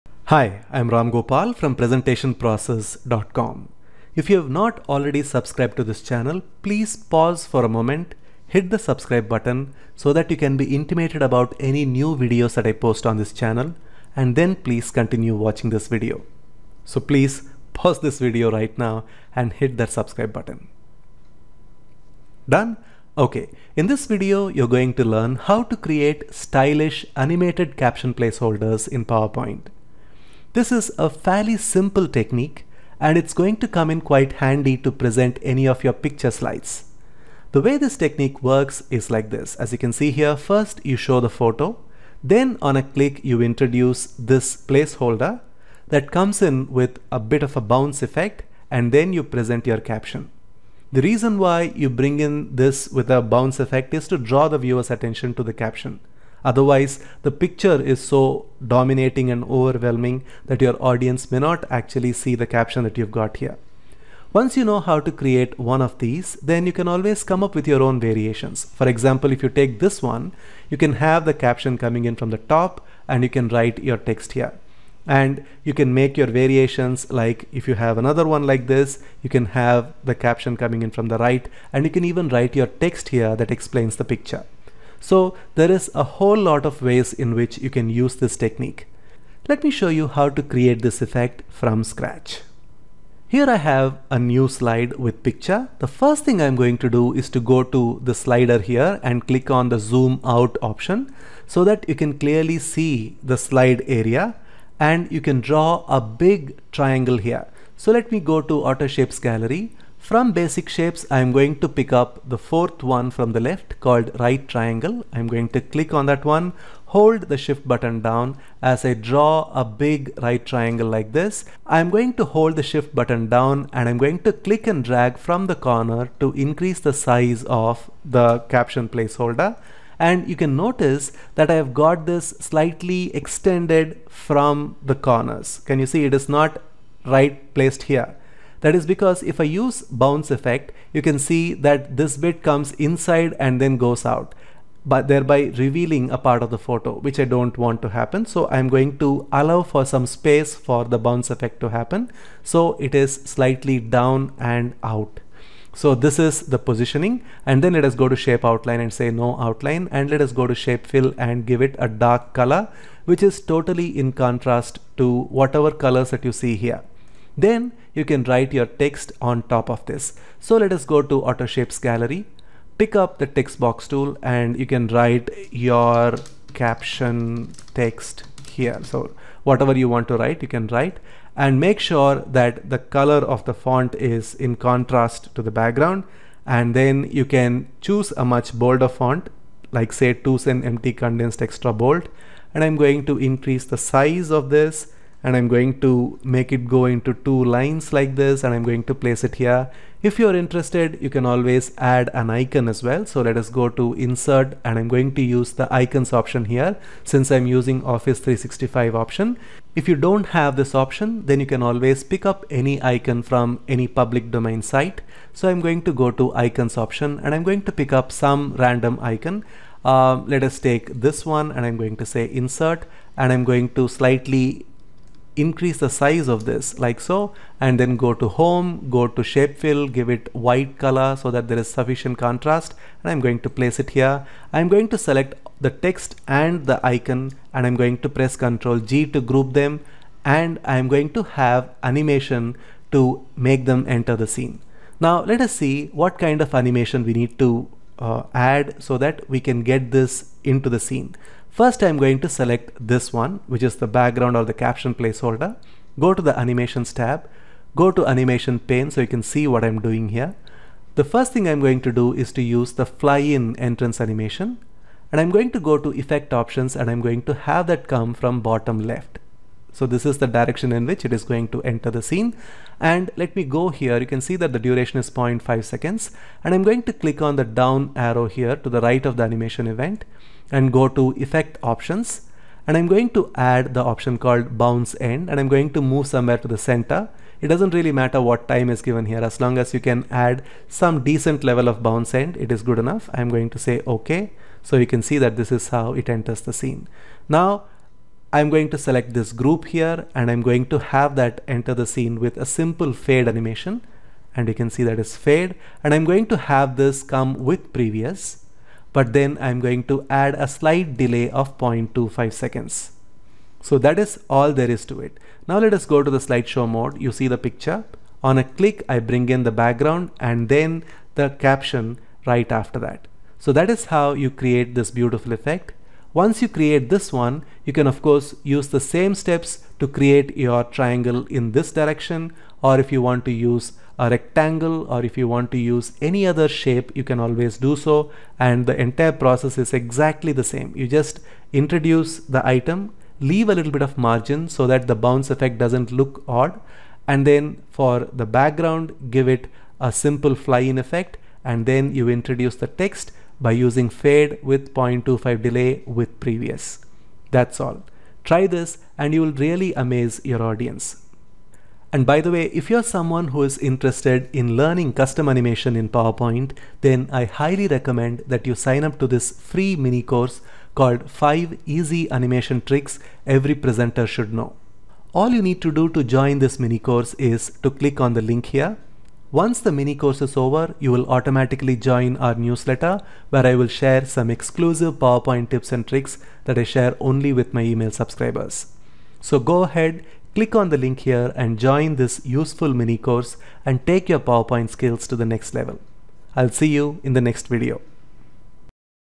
Create Cool Animated Placeholders for Captions : PowerPoint Animation
Learn how to create Animated Placeholders for Captions in PowerPoint. This is a simple technique and is handy to present any picture slides.

We add a bounce effect animation to the placeholder to draw viewers attention to the text.
Once you know how to create one, you can add your own variations in different colors, effects and styles.

You can follow this PowerPoint Animation tutorial in any version of PowerPoint 2010, 2013 or 2016. The video is created in PowerPoint on Office 365 by Ramgopal.

Advanced Animations Pack: 750 ready to Use templates